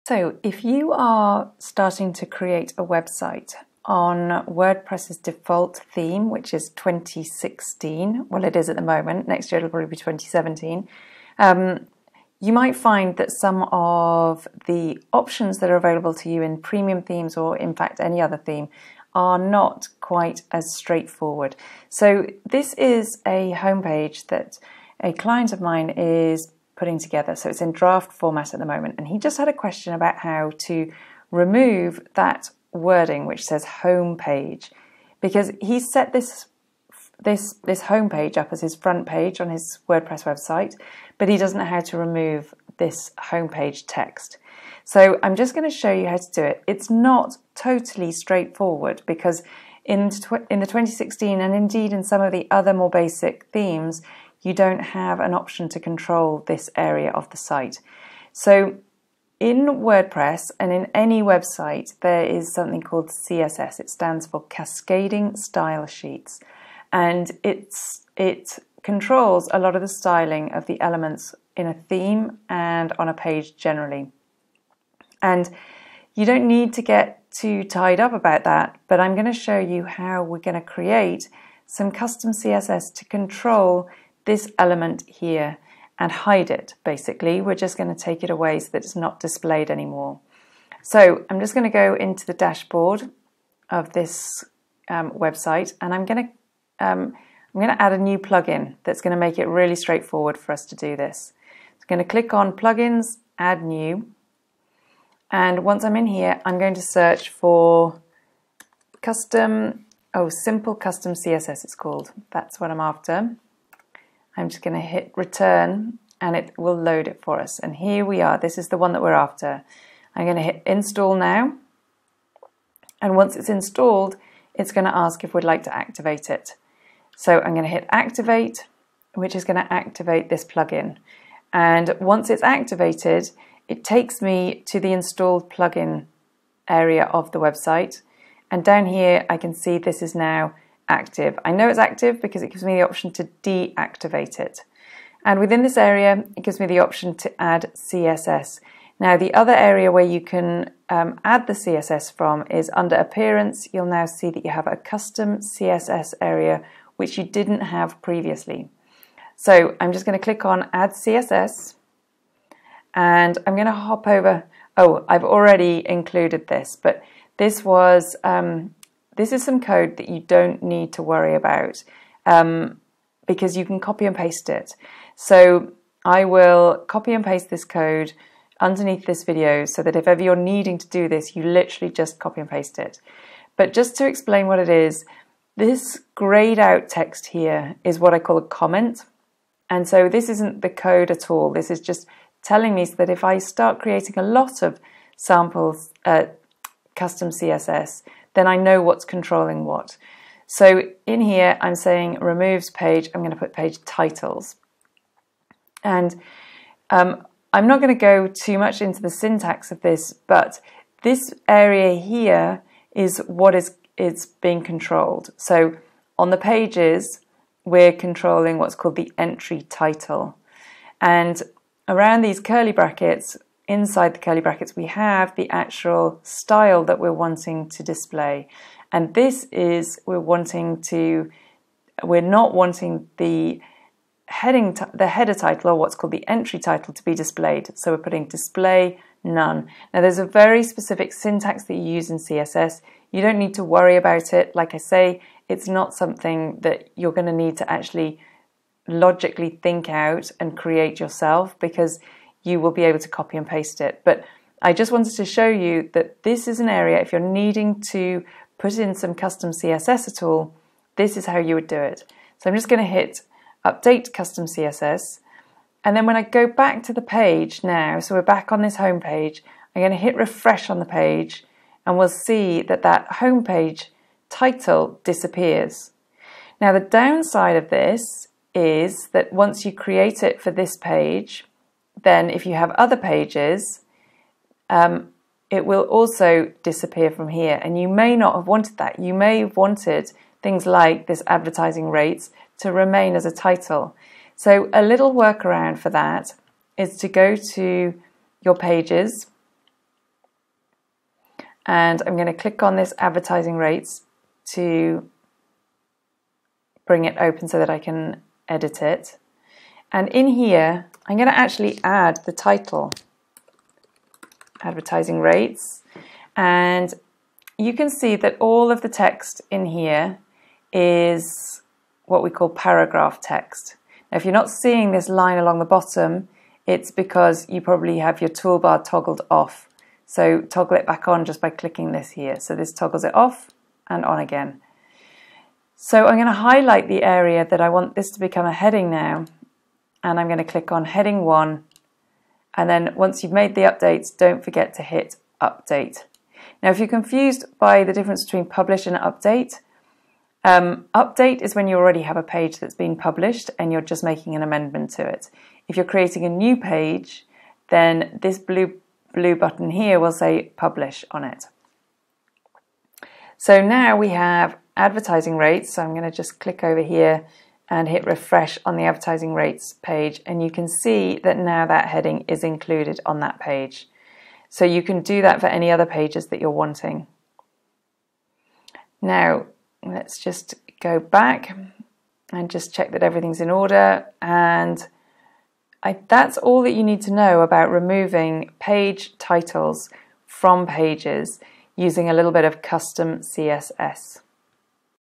Remove Page Titles in WordPress
Remove the Home title from your homepage on WordPress in a few easy steps. Use the simple custom CSS plugin and add the this custom css:
.page .type-page .entry-title {
    display: none;
No need to know coding/CSS - you can just copy and paste. This video will show you how to do this in a couple of simple steps, as well as how to reinstate titles on pages where you do want them displayed.

0:00 How to remove page titles
1:45 Custom CSS plugin

or join me in the WordPress Happy Community to join lots of other entrepreneurs DIYing their websites.